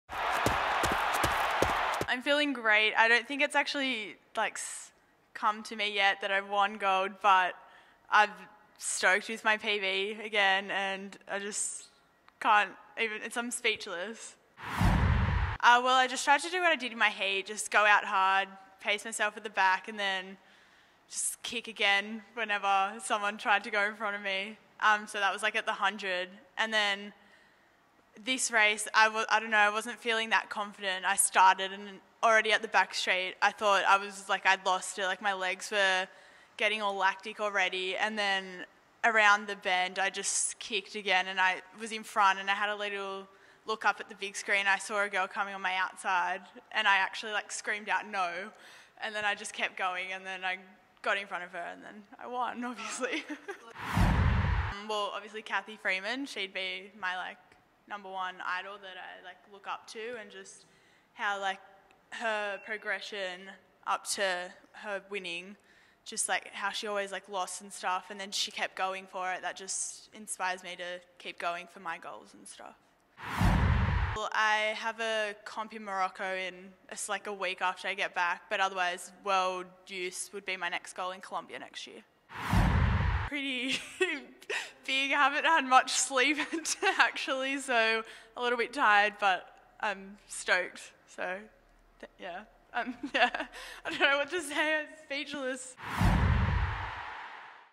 Jessica Thornton wins gold
Thornton claims the first athletics gold medal for Australia winning the 400 metres on day 7 of the 2014 Nanjing Youth Olympic Games.